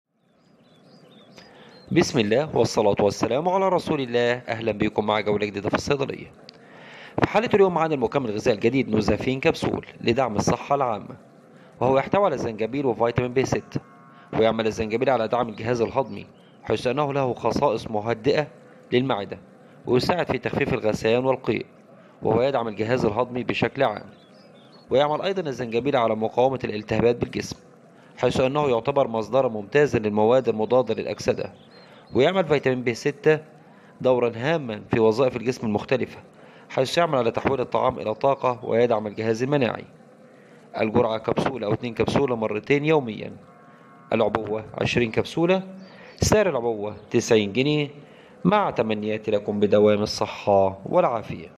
ممتاز لصحة الجهاز الهضمي ومضاد للالتهابات ومضاد للاكسدة وفوائد كتيييييير
اسم الصنف
نوزافين كبسول
Nuzaven Capsules

Ginger root     250mg

vitamin B6    25mg